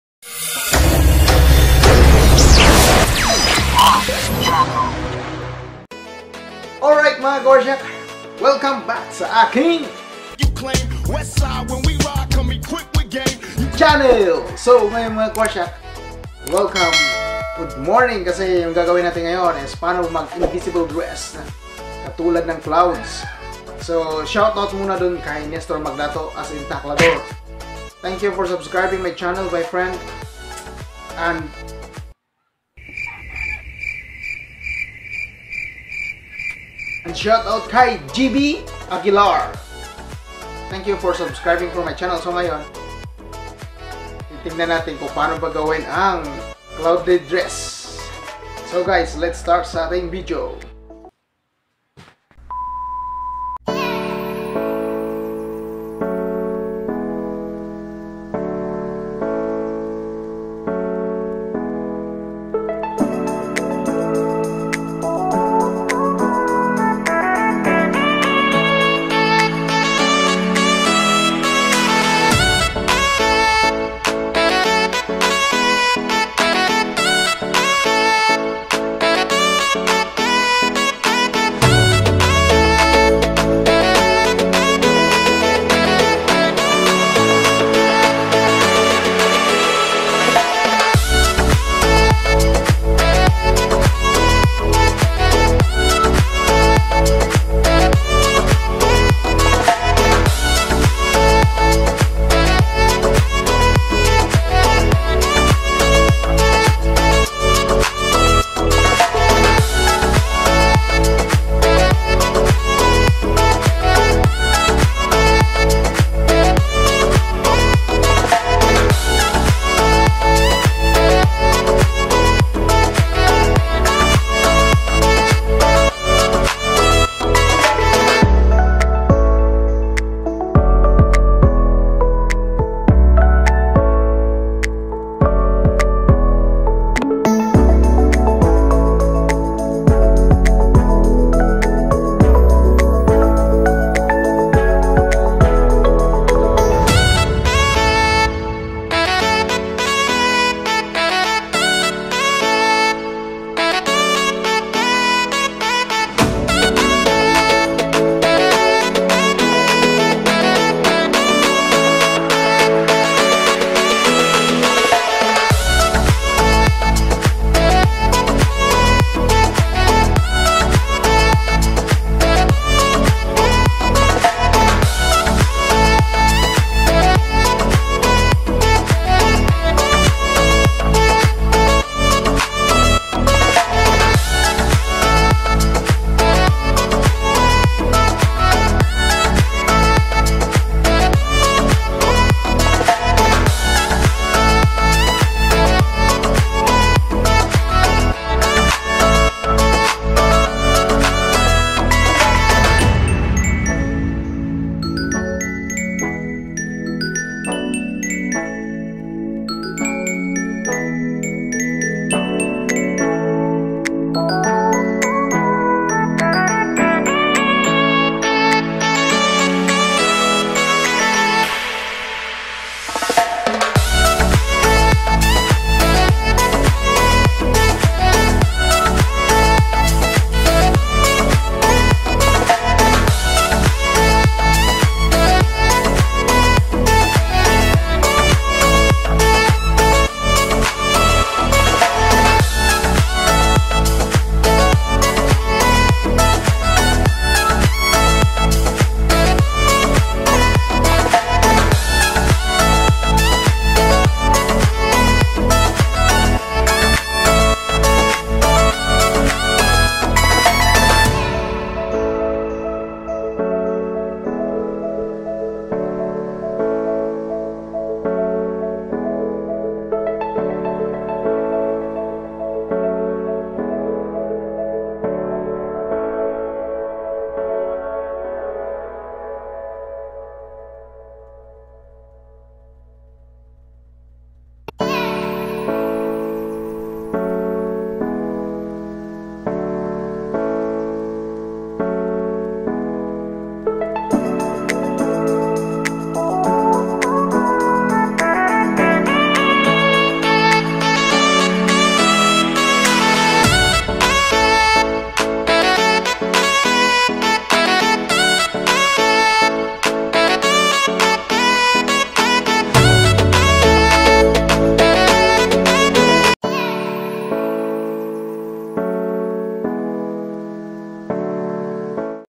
Transform Photos into Cloudy Dress: in Photo Shop(Nawala ang damit Grabi)
ito po ay simpling edit kung paano e dissolved and damit gamit lamang po ang Photo Shop ang Photo Shop po ay maganda gamitin kung kayo ay nag eedit kasi lahat nang tool nan dyan na.